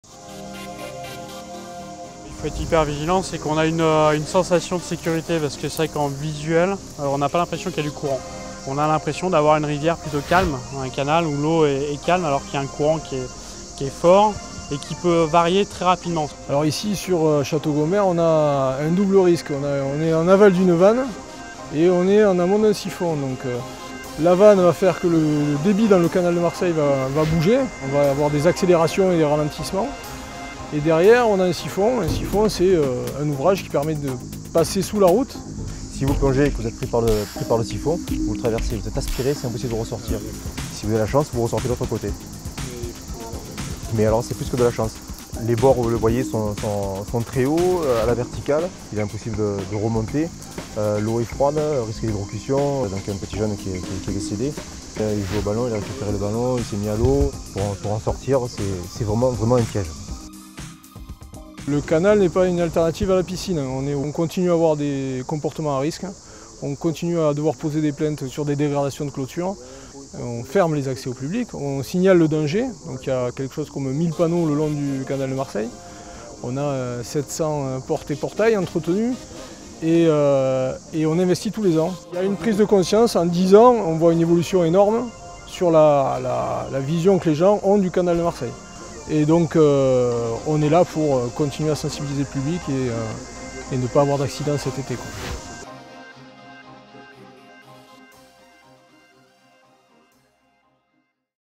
Danger ! Ne laisser pas les enfants s'approcher du canal de Marseille
Si aucun accident mortel n’est à déplorer depuis 12 ans, la Métropole Aix-Marseille Provence et la Société Eau de Marseille Métropole relancent une campagne de prévention sur la sécurité et les règles à respecter autour du canal car cet ouvrage peut s’avérer très dangereux. C’est pourquoi il est interdit de s’y promener et bien entendu de s’y baigner.

: Canal de Marseille : attention aux risques de noyades.

©  Eau de Marseille Métropole_2017